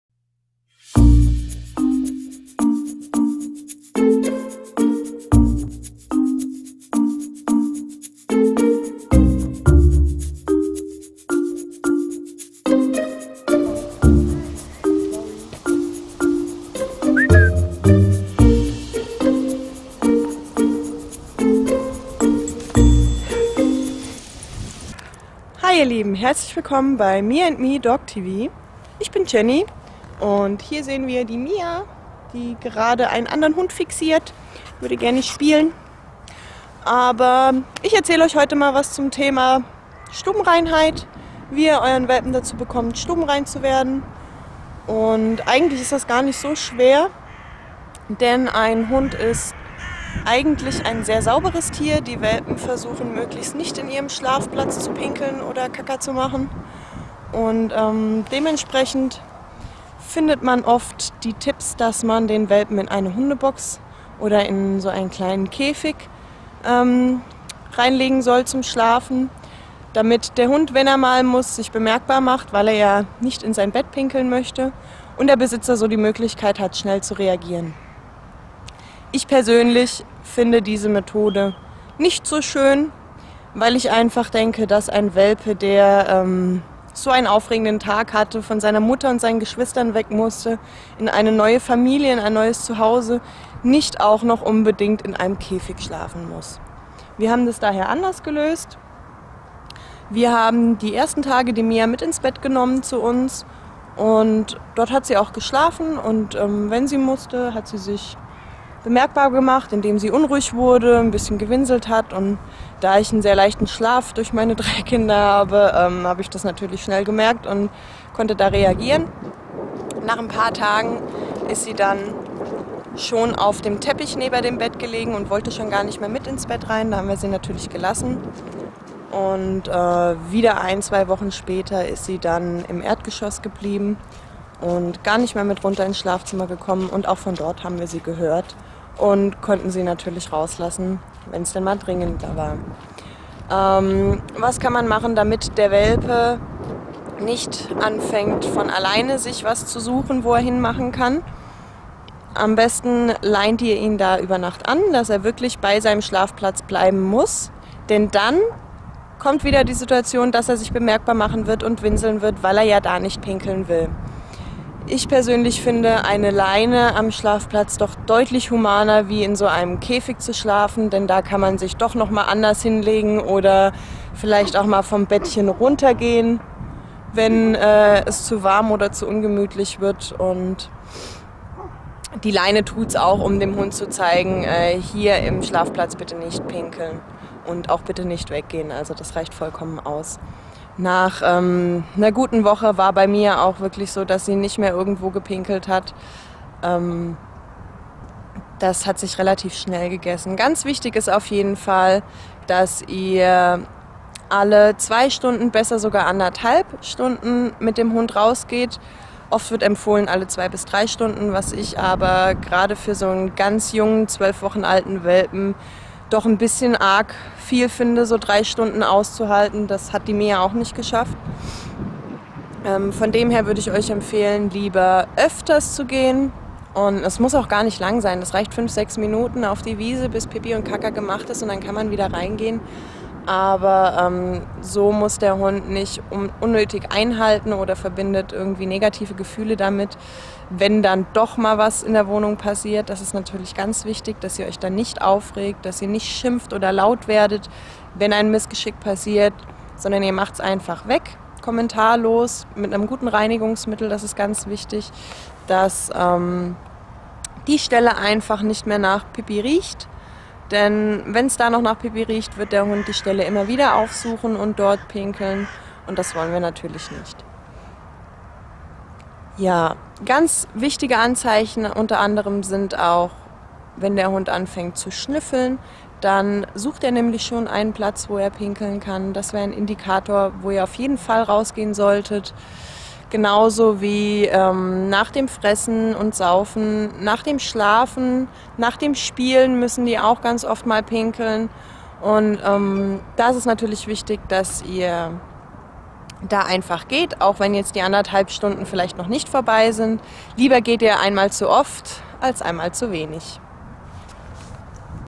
Welpenschule ►► Welpe stubenrein bekommen ✔ So wird dein Welpe schnell stubenrein.
Du willst wissen wie du deinen Hund dazu bekommst, sein Geschäft nicht (mehr) in der Wohnung zu machen?
Dann hab ich hier die Rettung für dich.
So wird dein Hund stubenrein.